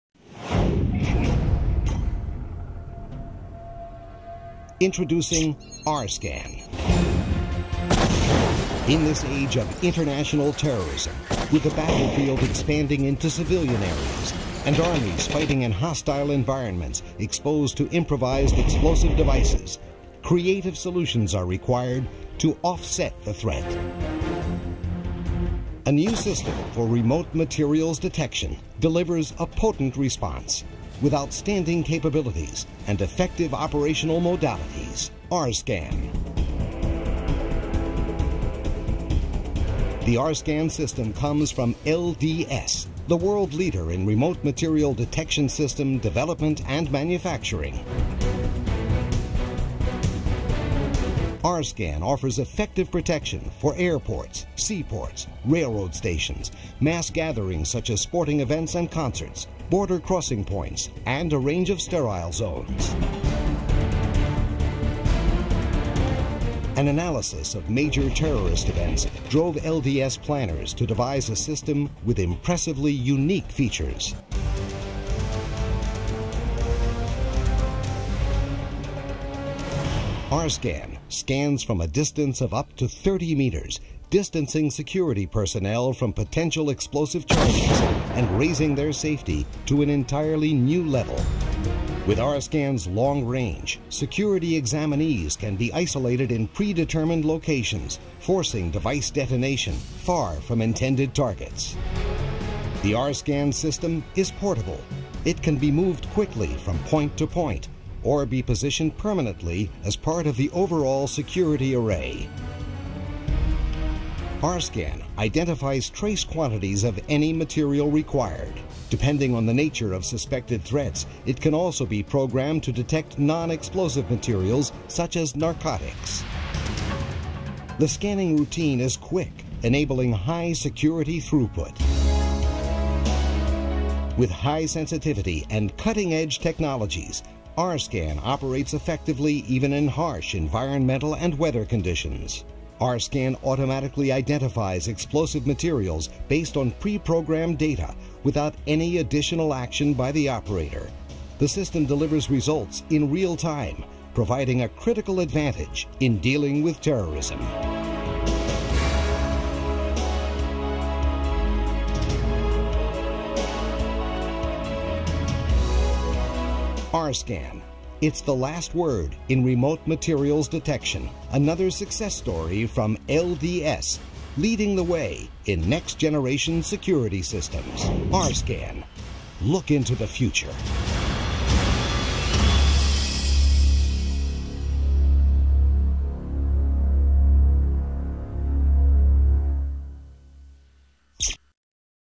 LDS Remote standoff Explosives Detection Laser Scanner
LDS Remote Scan (R-Scan)  presents a new approach in remote standoff explosives detection of Improvised Explosive Devices (IED's).